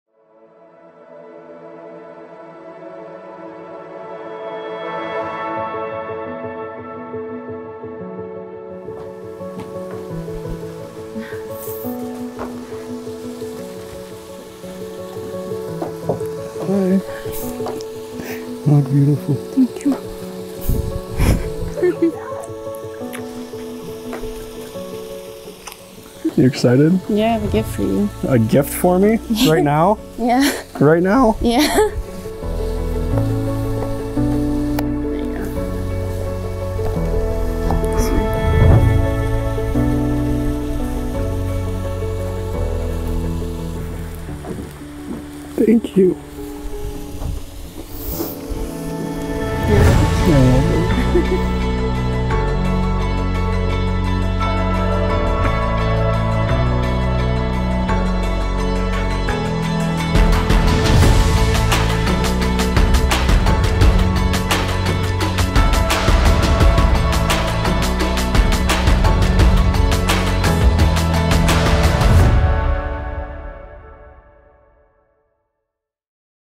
First Look and Groom Cries on their Wedding Day - Ivy and Drew's Film Teaser
I'm not crying, you're crying 😭❤️

Little tease from Ivy and Drew's wedding a few days ago. Love it!!! Can't wait to show the rest!

Congratulations guys!